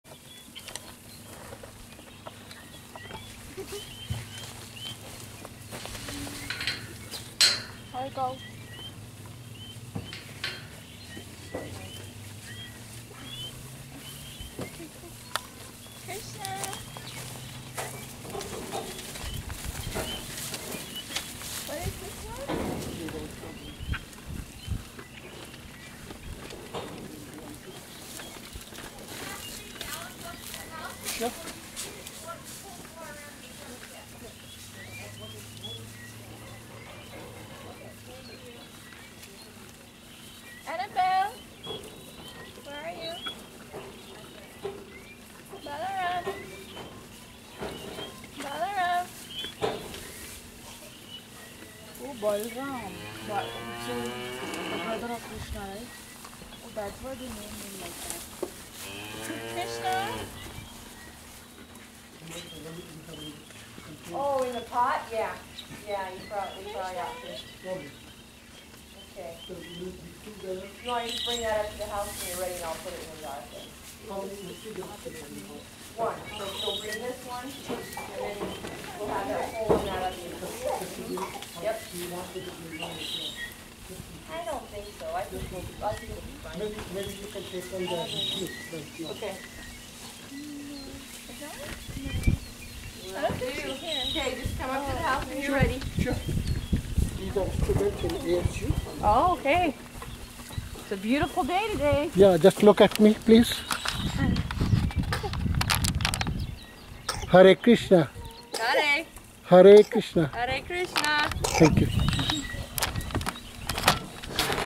GOSHALA ASU MAR 15 2015 020
Yola Wongk and Antara D Dave from Arizona State University visited GOSHALA. Both fed banana and hay besides brushing their bodies  to cows Annabel and Laila Rani, and two oxen Balarama and Krishna Das. We shared Ahimsa Milk and plucked oranges and grapefruits too.